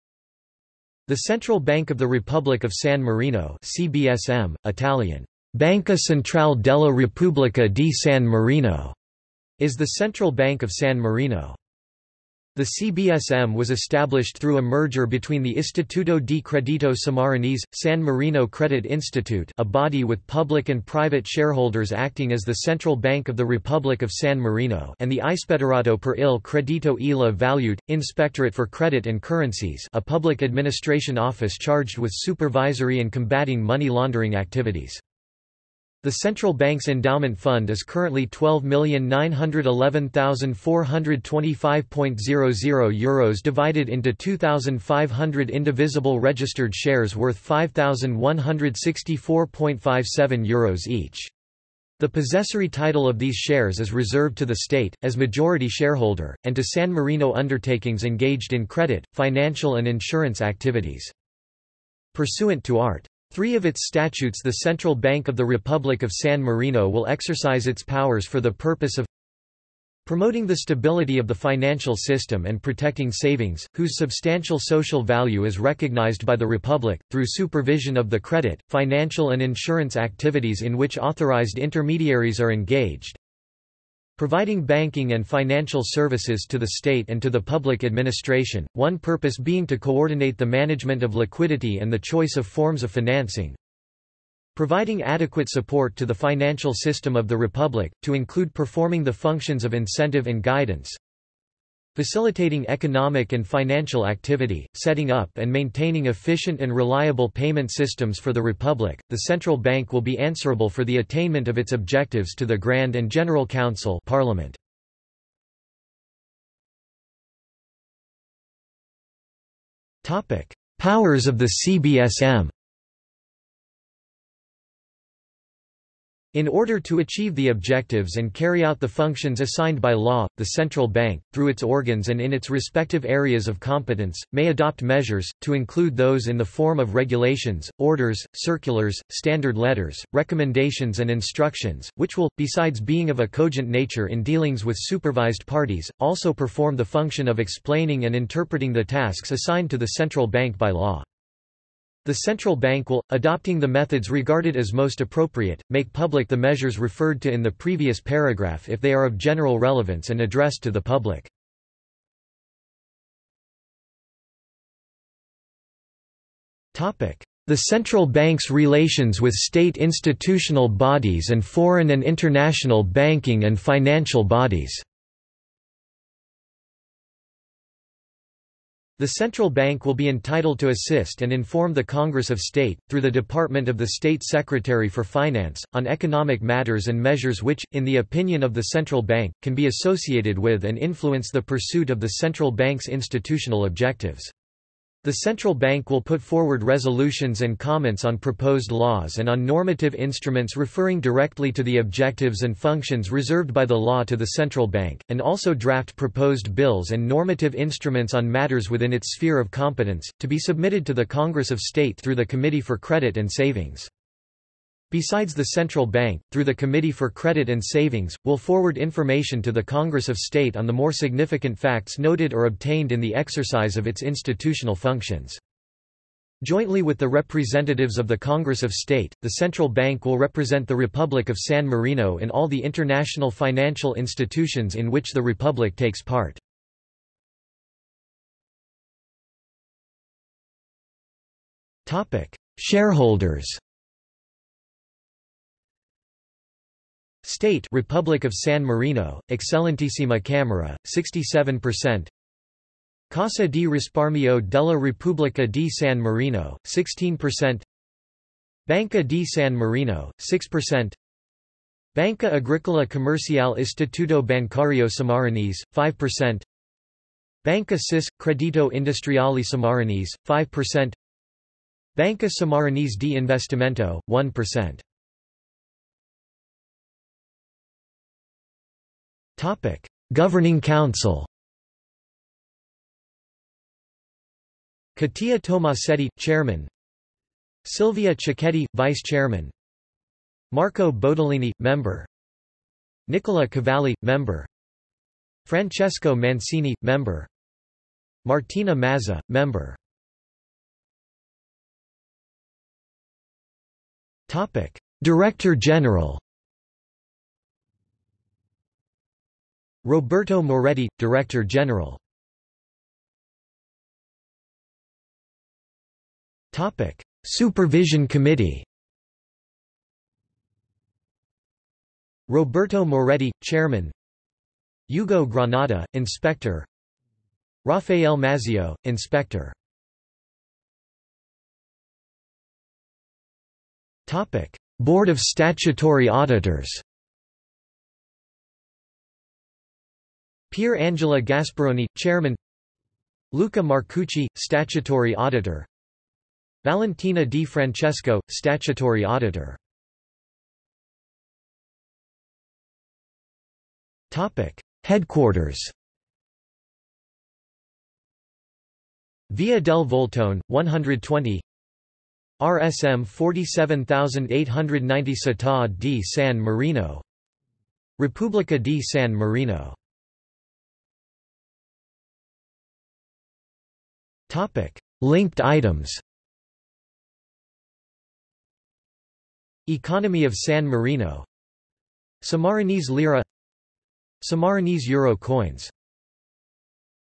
National bank of San Marino | Wikipedia audio article
00:01:56 1 Powers of the CBSM
00:02:43 2 The Central Bank's relations with State institutional bodies and foreign and international banking and financial bodies
00:03:58 3 Shareholders
00:04:35 4 Governing Council
00:05:00 5 Director General
00:05:12 6 Supervision Committee
00:05:30 7 Board of Statutory Auditors
00:05:50 8 Headquarters
00:06:10 9 Linked items

- Socrates

SUMMARY
The Central Bank of the Republic of San Marino (CBSM), (Italian "Banca Centrale della Repubblica di San Marino"), is the central bank of San Marino.
The CBSM was established through a merger between the Istituto di Credito Sammarinese – San Marino Credit Institute - (a body with public and private shareholders acting as the central bank of the Republic of San Marino) and the Ispettorato per il Credito e le Valute - Inspectorate for Credit and Currencies - (a Public Administration Office charged with supervisory and combating money-laundering activities).
The Central Bank’s endowment fund is currently 12,911,425.00 Euros divided into 2,500 indivisible registered shares worth 5,164.57 Euros each. The possessory title of these shares is reserved to the State, as majority shareholder, and to San Marino undertakings engaged in credit, financial and insurance activities.
Pursuant to art. 3 of its Statutes the Central Bank of the Republic of San Marino will exercise its powers for the purpose of:

promoting the stability of the financial system and protecting savings, whose substantial social value is recognised by the Republic, through supervision of the credit, financial and insurance activities in which authorised intermediaries are engaged;
providing banking and financial services to the State and to the Public Administration, one purpose being to coordinate the management of liquidity and the choice of forms of financing;
providing adequate support to the financial system of the Republic, to include performing the functions of incentive and guidance;
facilitating economic and financial activity, setting up and maintaining efficient and reliable payment systems for the Republic.The Central Bank will be answerable for the attainment of its objectives to the Grand and General Council (Parliament).